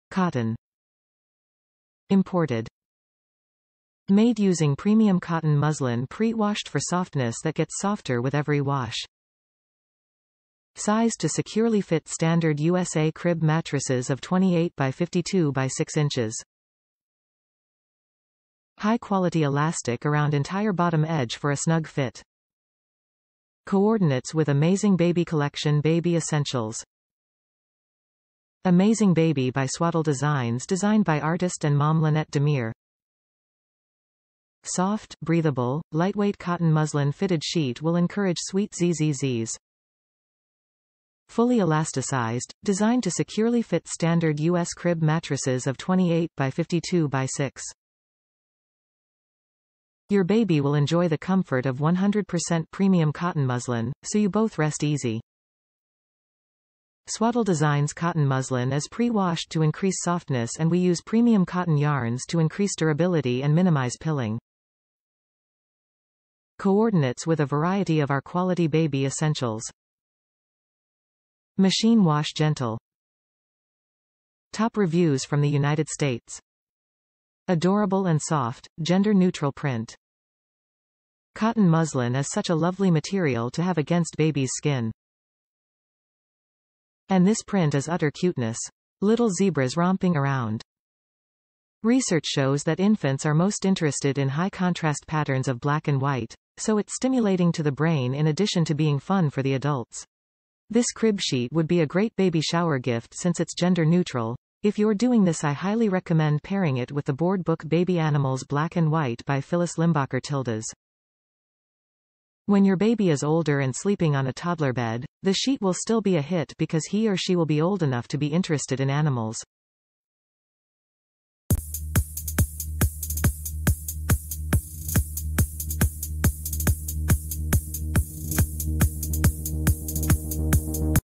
Amazing Baby Cotton Muslin Fitted Crib Sheet, Brushstroke, Denim
Cotton
Imported
Made using premium cotton muslin pre-washed for softness that gets softer with every wash
Sized to securely fit standard USA crib mattresses of 28 x 52 x 6 inches
High-quality elastic around entire bottom edge for a snug fit
Coordinates with Amazing Baby Collection baby essentials
Amazing Baby by SwaddleDesigns designed by artist and mom Lynette Damir
Soft, breathable, lightweight cotton muslin fitted sheet will encourage sweet zzZs. Fully elasticized, designed to securely fit standard US crib mattresses of 28 x 52 x 6". Your baby will enjoy the comfort of 100% premium cotton muslin, so you both rest easy. Swaddledesigns cotton muslin is pre-washed to increase softness and we use premium cotton yarns to increase durability and minimize pilling. Coordinates with a variety of our quality baby essentials. Machine wash gentle.

Adorable and soft, gender-neutral print
Cotton muslin is such a lovely material to have against baby's skin. And this print is utter cuteness! Little zebras romping around. Research shows that infants are most interested in high contrast patterns of black and white, so it's stimulating to the brain in addition to being fun for the adults! This crib sheet would be a great baby shower gift since it's gender neutral (if you're doing this I highly recommend pairing it with the board book Baby Animals Black and White by Phyllis Limbacher Tildes). When your baby is older and sleeping on a toddler bed, the sheet will still be a hit because he or she will be old enough to be interested in animals!